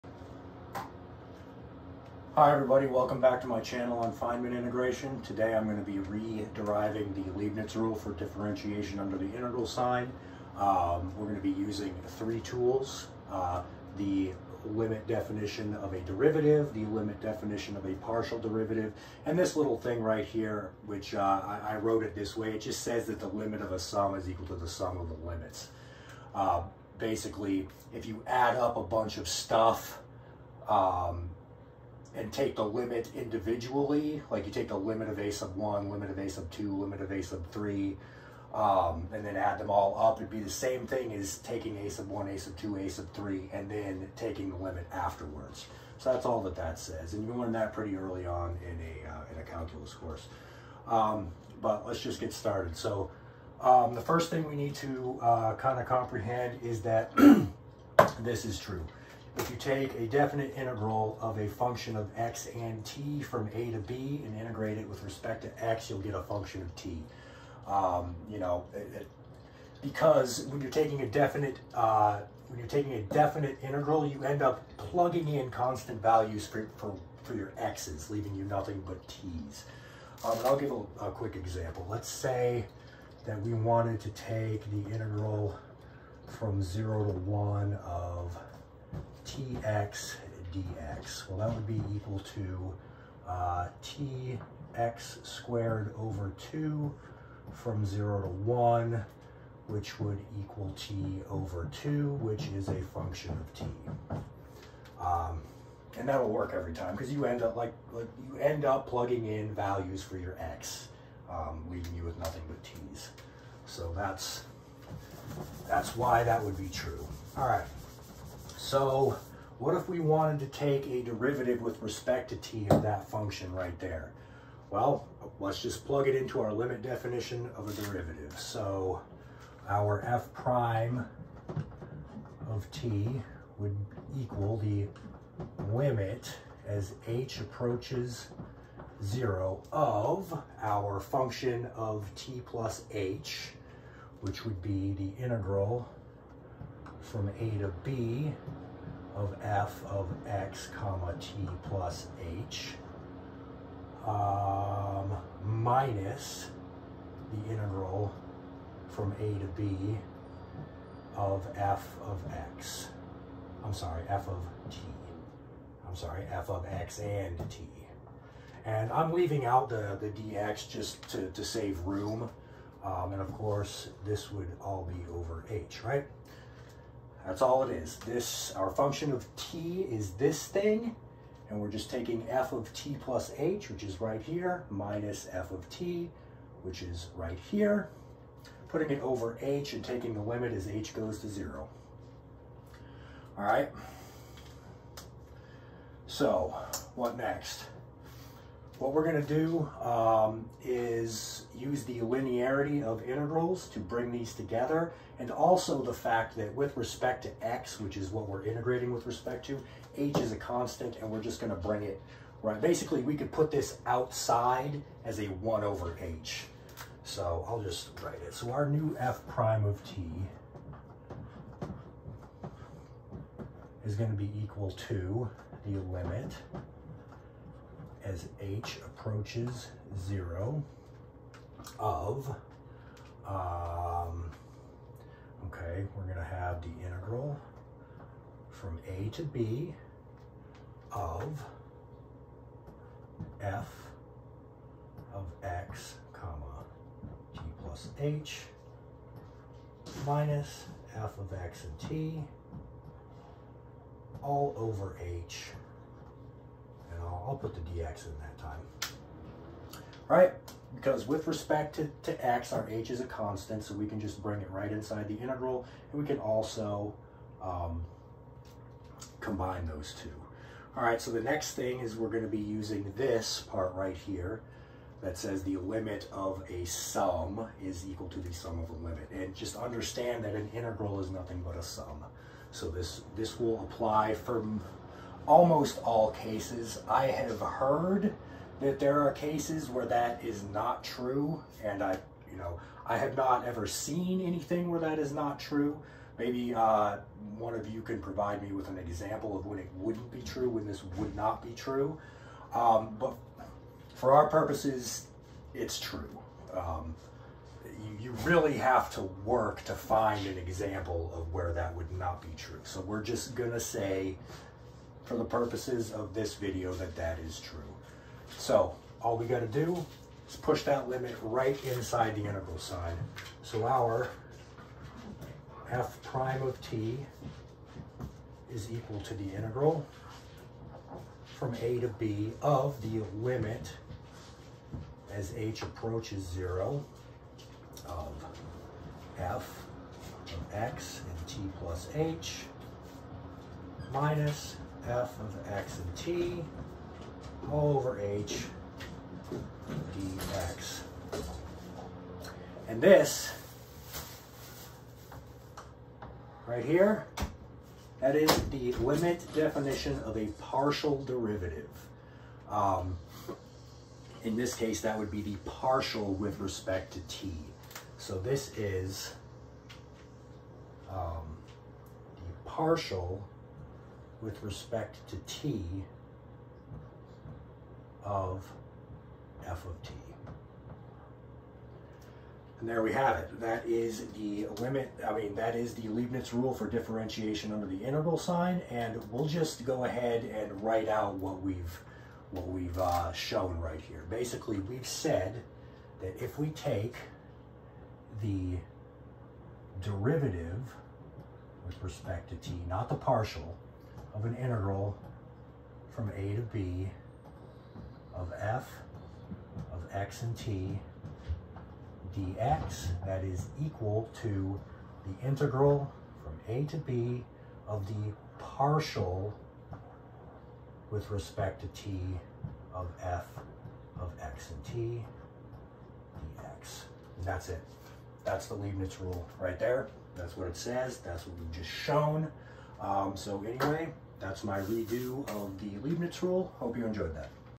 The Leibniz rule for differentiation under the integral sign - Lecture
Derivation of the Leibniz rule for differentiation under the integral sign